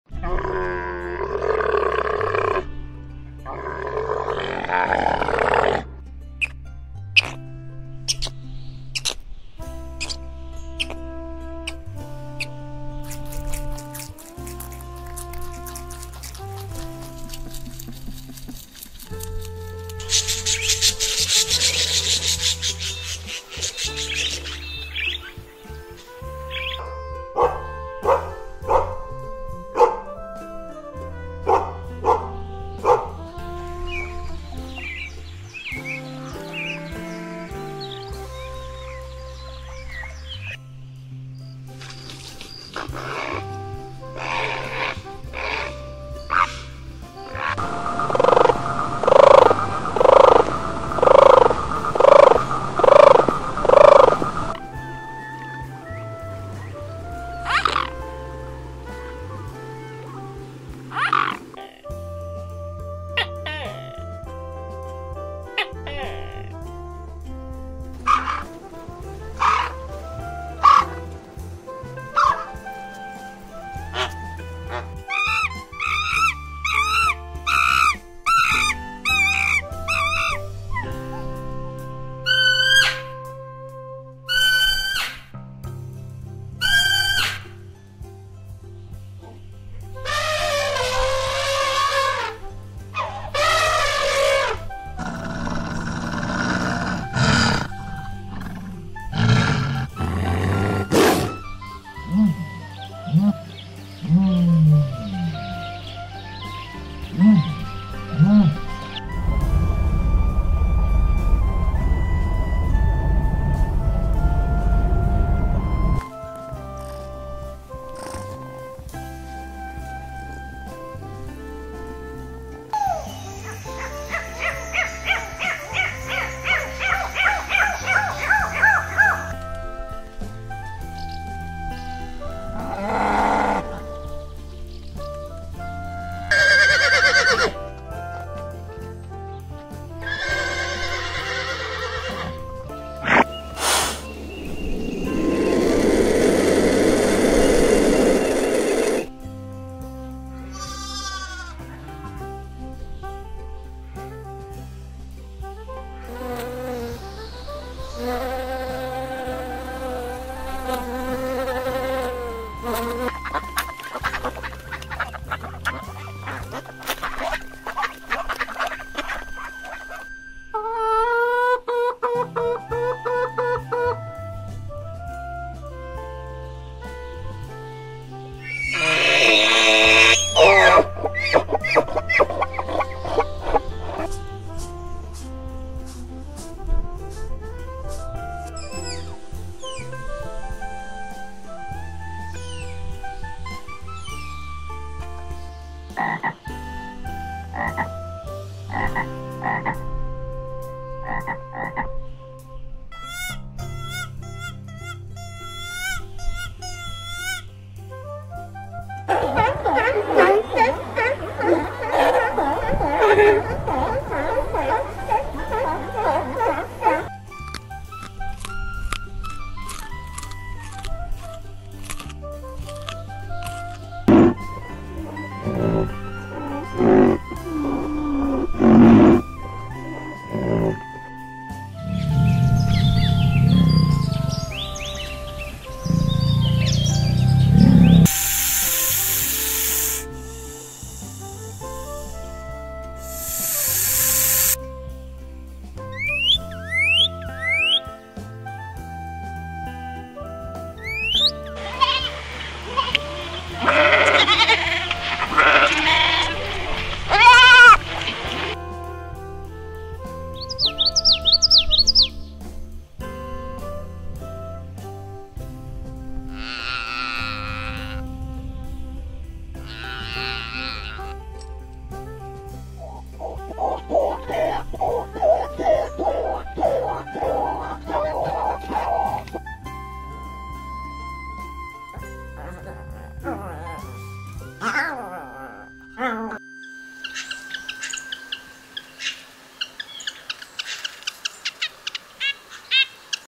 To hear the sounds of cute. animals,monkey,dog,bear,seal,the elephant,cat,a duck,horse,Tiger,,2023
Animals live with us for a very long time. So let's take a look and listen them. Animals make very lively and interesting sounds. Each species has a different sound. Let's listen and learn about the sounds of animals.